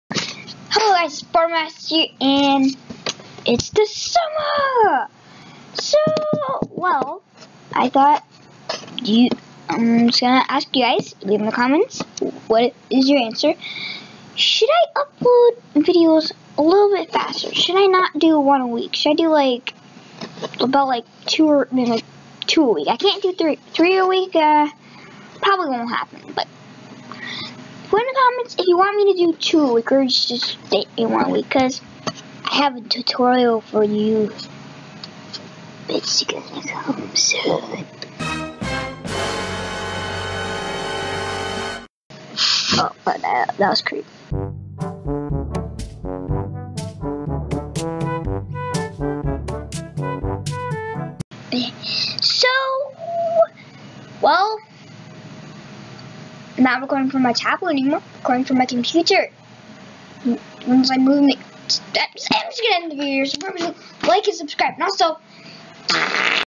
Should I Speed Up My Upload Speed?
In This Video I Ask If I Should Upload 2 Videos A Week